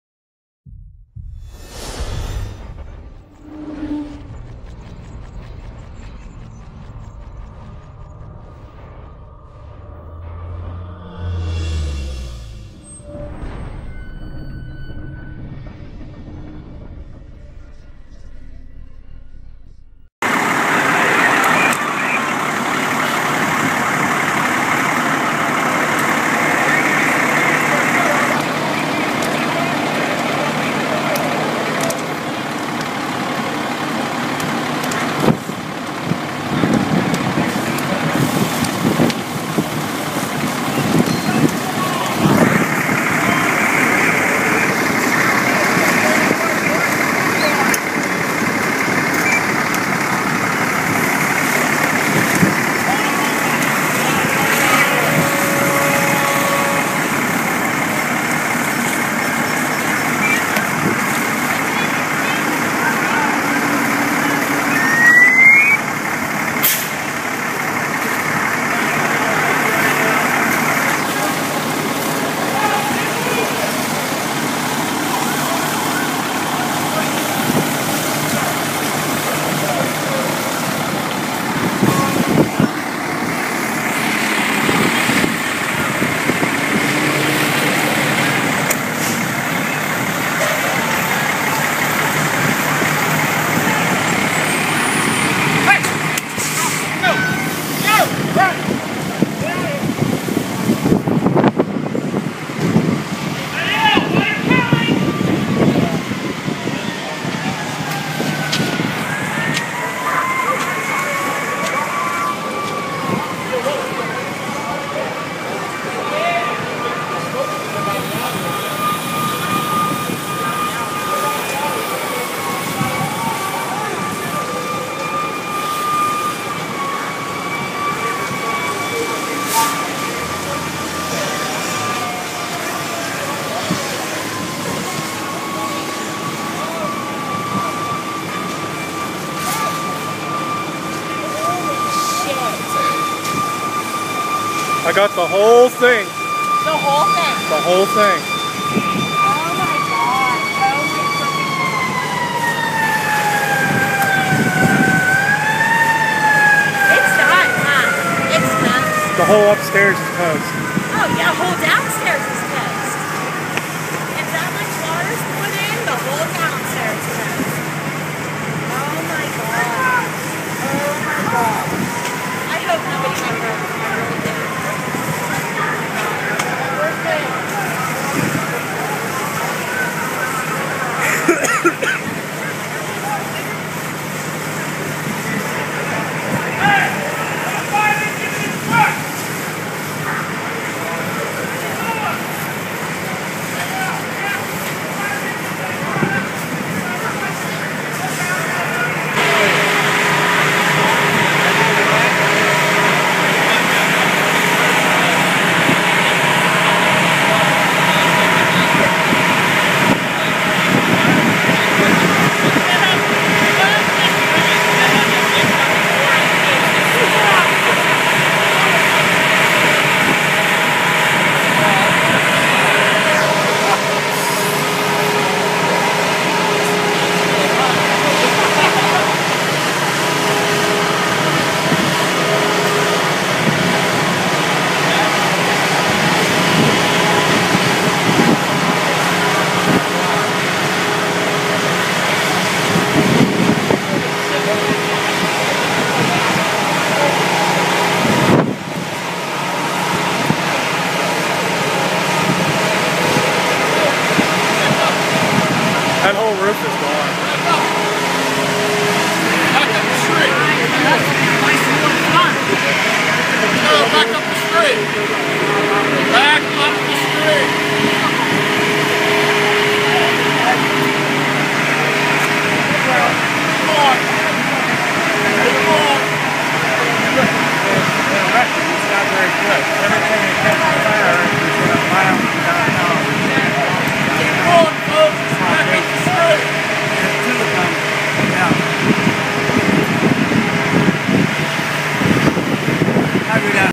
Latrobe Pa - Ligonier Street Grub and Pub Fire - HD
BEST VIEWED IN 1080p - This fire damaged a lot of property

Filmed by: Nicholas Molitor

 All Fire footage here is the property of Iron Tusk Productions and Nicholas Molitor
 Intro made by Iron Tusk Productions, Template use was granted permission by Lionsgate Entertainment Corp.

to skip the intro go to 0:19

this video is 3D compatible.